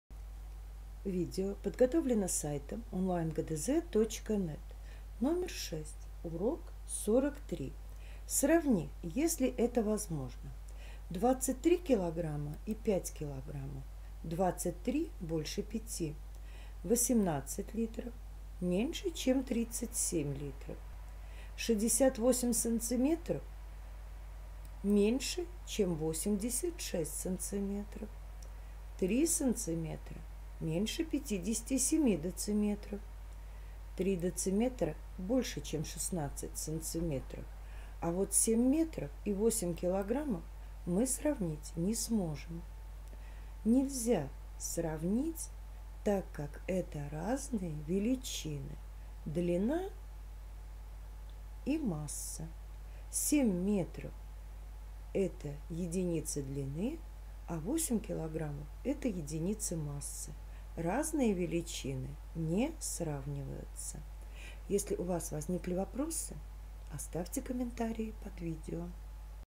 Урок 43 Задание 6 – ГДЗ по математике 1 класс (Петерсон Л.Г.) Часть 3
В данном номере сравниваем единицы длины, массы или объёма, если это возможно.